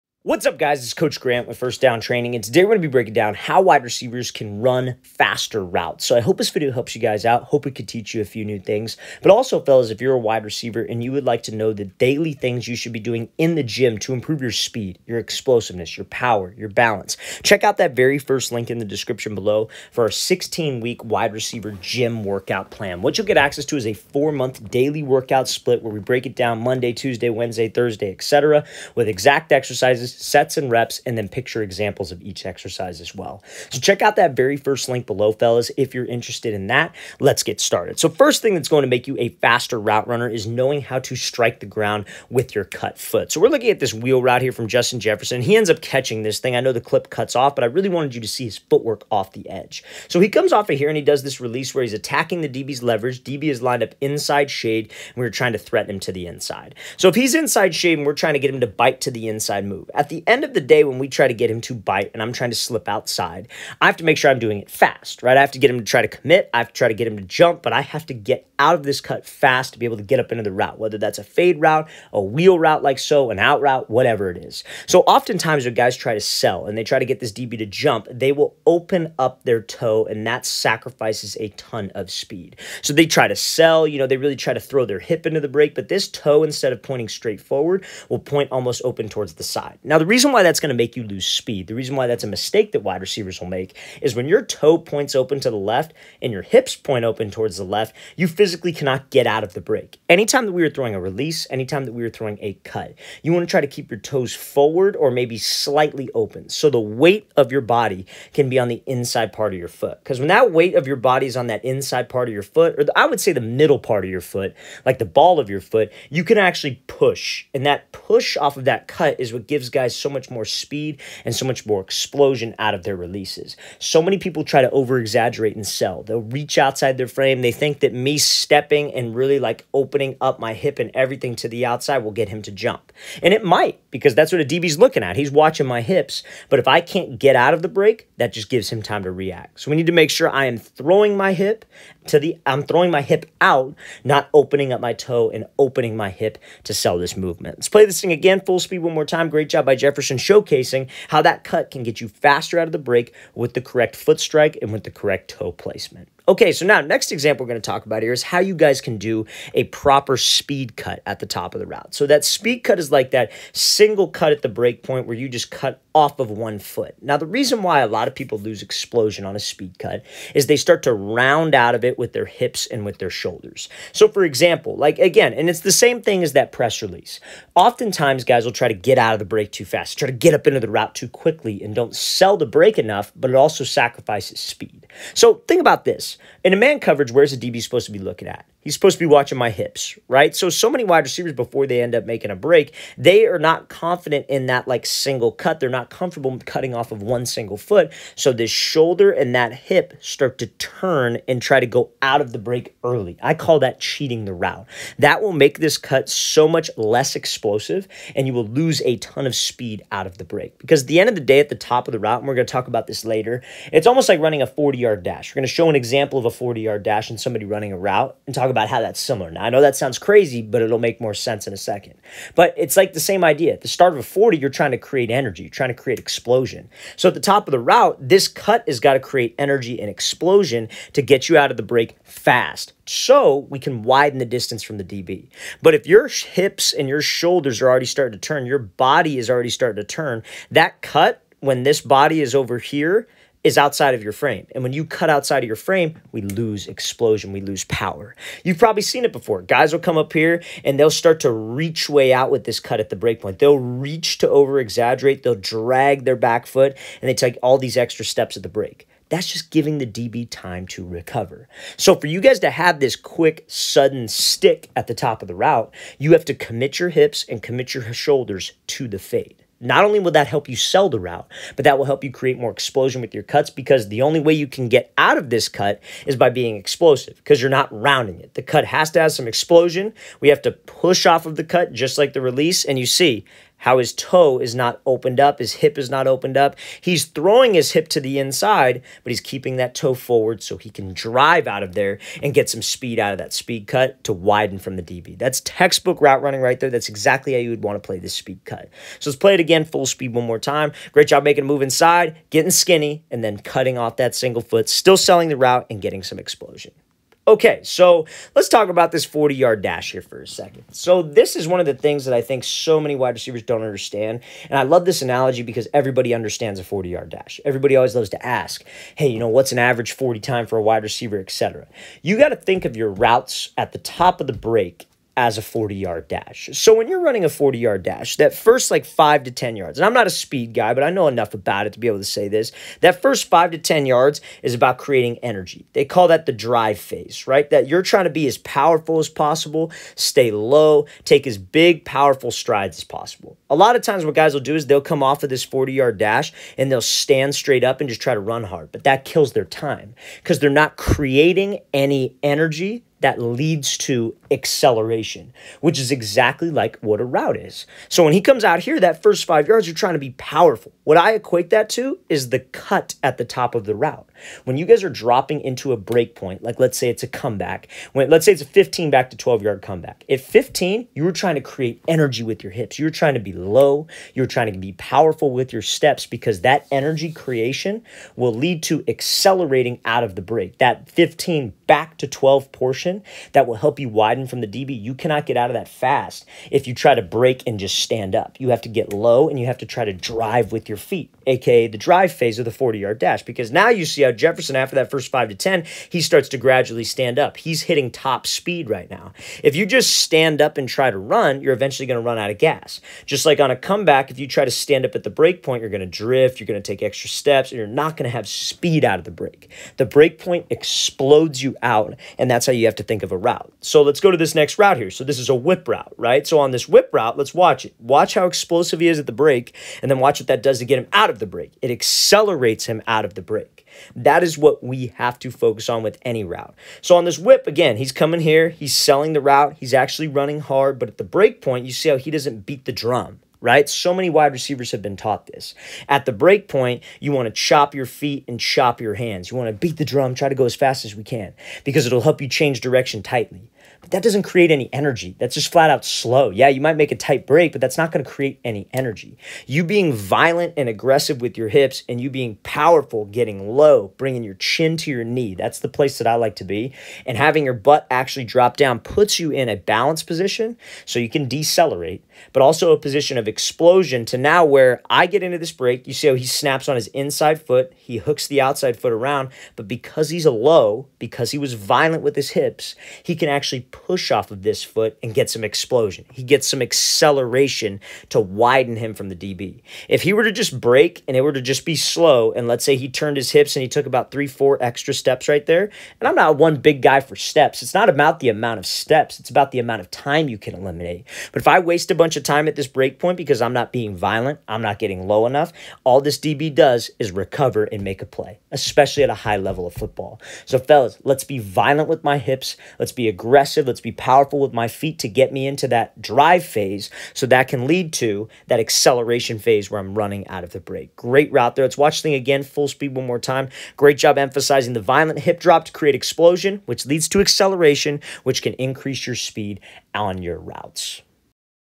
Watch This To Get WAY FASTER…
In this video I breakdown HOW WRs CAN RUN FASTER ROUTES. I hope you enjoy!